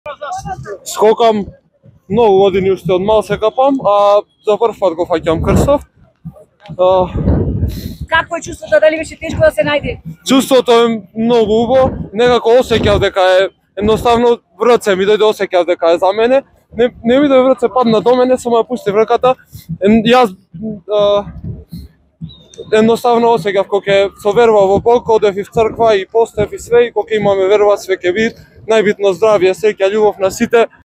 Васко Довлев Фаќач на крстот во Охрид
19 годишниот Васко Довлев од Варош го фати крстот што на празникот Богојавление Водици, денеска, беше положен во осветените води на Охридското Езеро. Тој беше најсреќен помеѓу неколкуте стотини капачи.